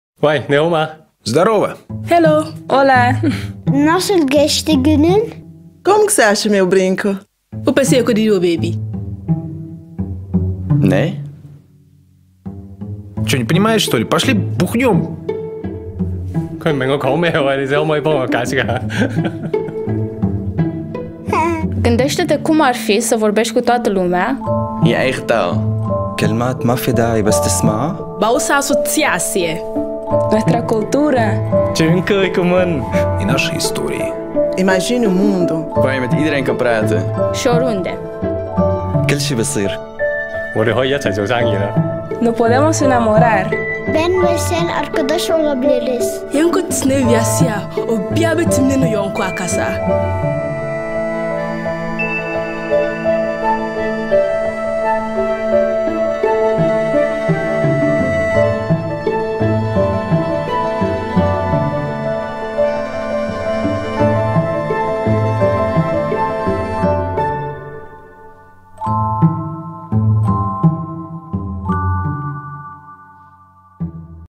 荷蘭Travis 80 種語言翻譯機 -AI 雙向 實時翻譯
Travis 是一款外觀像無線遙控器的翻譯裝置，大小可以剛好放入口袋中； Travis 團隊所希望達成的目標，是只要按下一個按鍵就能讓不同母語背景的人面對面溝通，背後的原理是透過 AI 技術幫忙，讓商旅過程溝通無礙。能夠自動辨識人聲來開啟對話的 Travis，在連上網路的情況下，可以翻譯從南非荷蘭語到威爾士語等高達 80 種語言，離網情況下則可以翻譯多達 20 種語言；Travis 上有一塊觸控螢幕，搭配一個四核處理器，可以連結 Wi-Fi 、藍芽及一個 SIM 卡插槽，充飽電的情況下可以持續待機使用達一周，連續使用可達 12 小時。